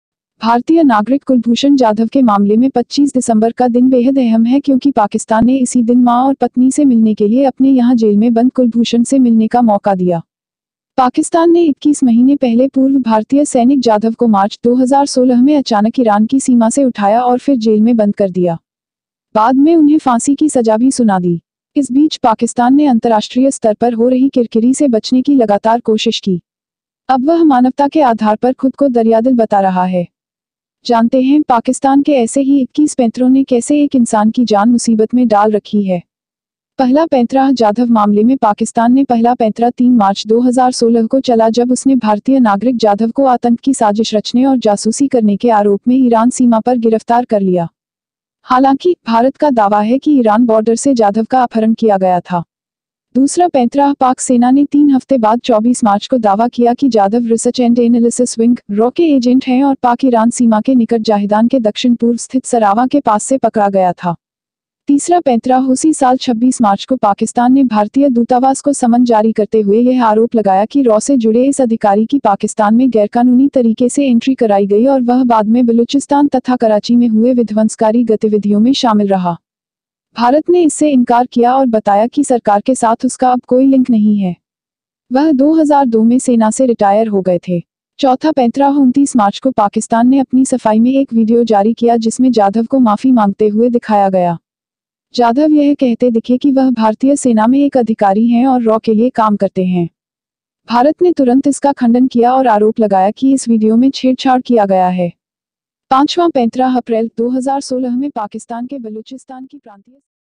जाधव केस: 21 माह में पाकिस्तान ने आजमाए ये 21 पैंतरें
भारतीय नागरिक कुलभूषण जाधव के मामले में 25 दिसंबर का दिन बेहद अहम है क्योंकि पाकिस्तान ने इसी दिन मां और पत्नी से मिलने के लिए अपने यहां जेल में बंद कुलभूषण से मिलने का मौका दिया.

पाकिस्तान ने 21 महीने पहले पूर्व भारतीय सैनिक जाधव को मार्च, 2016 में अचानक ईरान की सीमा से उठाया और फिर जेल में बंद कर दिया. बाद में उन्हें फांसी की सजा भी सुना दी. इस बीच पाकिस्तान ने अंतरराष्ट्रीय स्तर पर हो रही किरकिरी से बचने की लगातार कोशिश की. अब वह मानवता के आधार पर खुद को दरियादिल बता रहा है. जानते हैं पाकिस्तान के ऐसे ही 21 पैंतरों ने कैसे एक इंसान की जान मुसीबत में डाल रखी है.

पहला पैंतराः जाधव मामले में पाकिस्तान ने पहला पैंतरा 3 मार्च, 2016 को चला जब उसने भारतीय नागरिक जाधव को आतंक की साजिश रचने और जासूसी करने के आरोप में ईरान सीमा पर गिरफ्तार कर लिया. हालांकि, भारत का दावा है कि ईरान बॉर्डर से जाधव का अपहरण किया गया था.

दूसरा पैंतराः पाक सेना ने 3 हफ्ते बाद 24 मार्च को दावा किया कि जाधव रिसर्च एंड एनलिसिस विंग (रॉ) के एजेंट हैं और पाक-ईरान सीमा के निकट जाहिदान के दक्षिण-पूर्व स्थित सरावां के पास से पकड़ा गया था.

तीसरा पैंतराः उसी साल 26 मार्च को पाकिस्तान ने भारतीय दूतावास को समन जारी करते हुए यह आरोप लगाया कि रॉ से जुड़े इस अधिकारी की पाकिस्तान में गैरकानूनी तरीके से एंट्री कराई गई और वह बाद में बलुचिस्तान तथा कराची में हुए विध्वंसकारी गतिविधियों में शामिल रहा. भारत ने इससे इंकार किया और बताया कि सरकार के साथ उसका अब कोई लिंक नहीं है. वह 2002 में सेना से रिटायर हो गए थे."

चौथा पैंतराः 29 मार्च को पाकिस्तान ने अपनी सफाई में एक वीडियो जारी किया जिसमें जाधव को माफी मांगते हुए दिखाया गया. जाधव यह कहते दिखे कि वह भारतीय सेना में एक अधिकारी हैं और रॉ के लिए काम करते हैं. भारत ने तुरंत इसका खंडन किया और आरोप लगाया कि इस वीडियो में छेड़छाड़ किया गया है.

पांचवां पैंतराः अप्रैल, 2016 में पाकिस्तान के बलुचिस्तान की प्रांतीय सरकार ने जाधव पर आतंक फैलाने और तोड़फोड़ की साजिश रचने के खिलाफ एफआईआर दर्ज कराया.

छठा पैंतराः पाकिस्तान (7 दिसंबर, 2016) ने जाधव मामले में फिर नया सुर साधा. अब पाकिस्तान के प्रधानमंत्री के विदेश मामलों के सलाहकार सरताज अजीज ने दावा किया कि जाधव पर दिए गए सबूत पर्याप्त नहीं हैं. जाधव पर मुहैया कराए गए डोशिएर महज बयानभर है.

सातवां पैंतराः 31 दिसंबर, 2016 को पाकिस्तान ने कहा कि वह भारतीय नागरिक जाधव से जुड़ा एक डोशिएर यूएन सचिव जनरल एंटोनियो गुटेरस को सौंपेगा. साथ ही पाकिस्तान ने सीमापार नियम तोड़ने के भारतीय प्रयास के सबूत भी दि